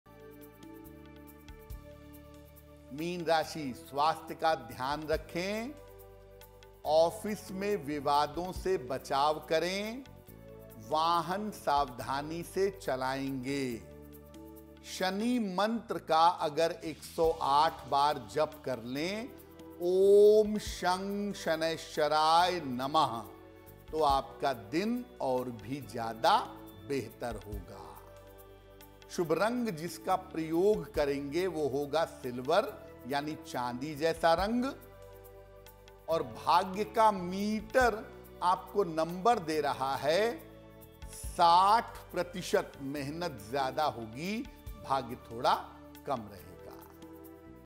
Aaj Ka Meen Rashifal 6 जनवरी 2024: वाहन सावधानी से चलाएं | Pisces Horoscope Today
Aaj Ka Rashifal Meen Rashi 6 जनवरी 2024: मीन राशि के जातकों का ज्योतिष के अनुसार स्वास्थ्य का ध्यान रखें. ऑफिस में विवादों से बचाव करें. वाहन सावधानी से चलाएं. शनि मंत्र का जाप करें. आज का शुभ रंग सिल्वर है.